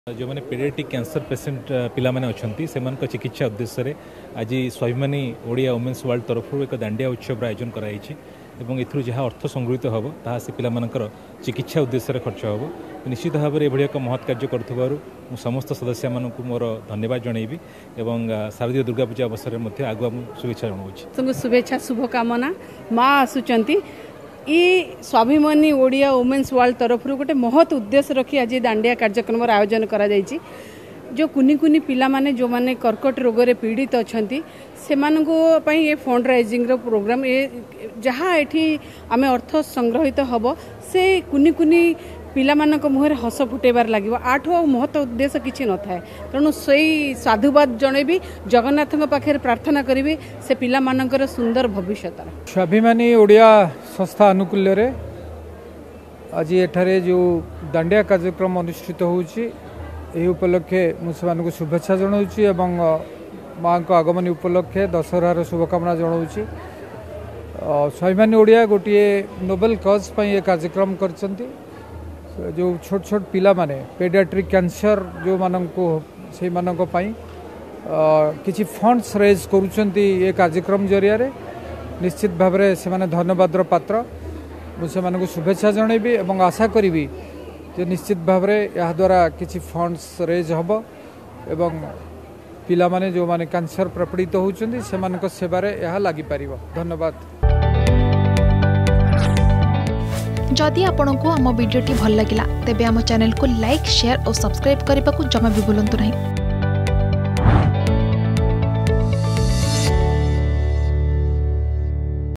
Dandia Dance Will Bring In Smiles On The Face Of Cancer-Affected Children
Dandia dance will bring in smiles on the face of cancer-affected children. All the income will go to the treatment of these kids. This is an initiative by Swabhimani Odia Women's World .।